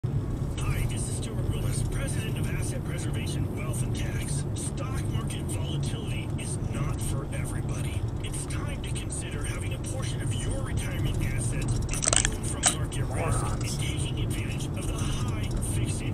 Pickup in center lane is NOT going to miss his turn
I go now.  Good luck you.  Pickup in center lane is NOT going to miss his turn, dammit!